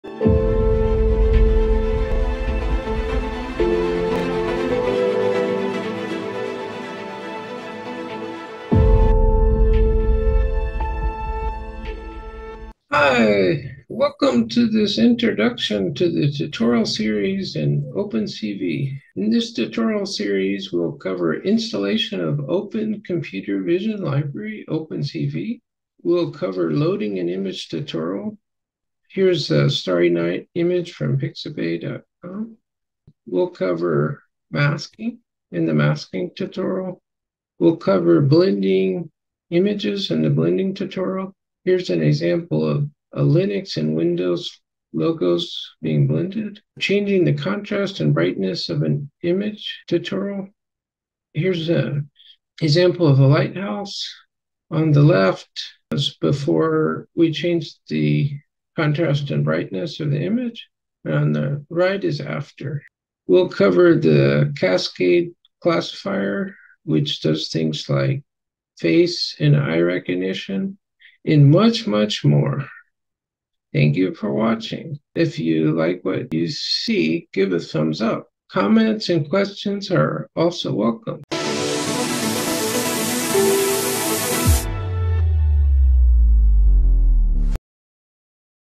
Trailer of Tutorials in Open Computer Vision Library (Open CV)
The is the trailer which describes all the Tutorials that will be provided on the Open Computer Vision Library (Open CV).